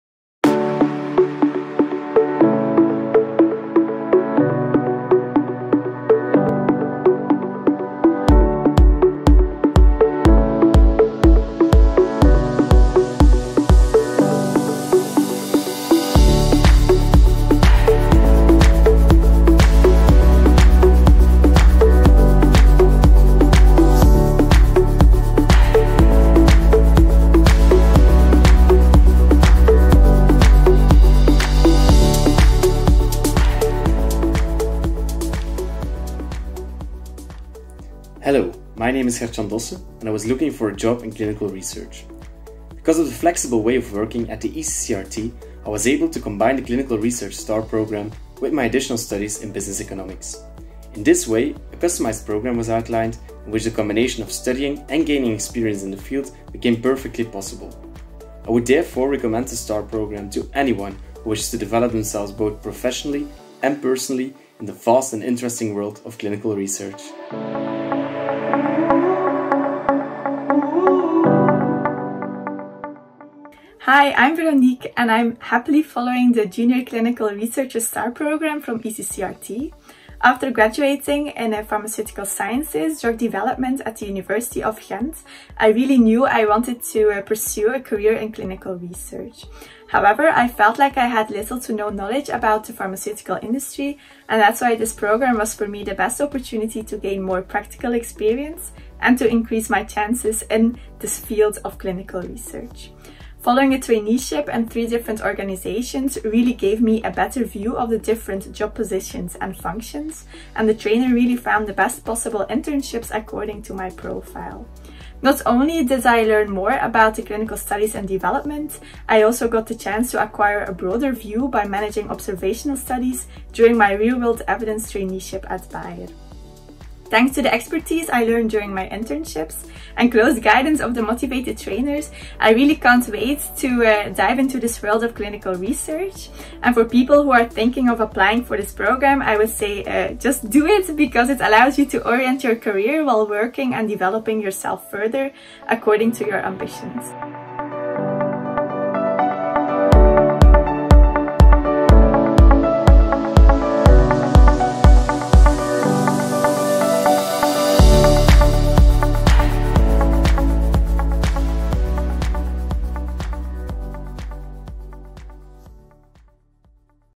Finding a job in clinical research - STAR Programme
Video content:
00:00 - Introduction
00:37 - Gert-Jan testimonial
01:13 - Veronique Shiwa testimonial
02:27 - Conclusion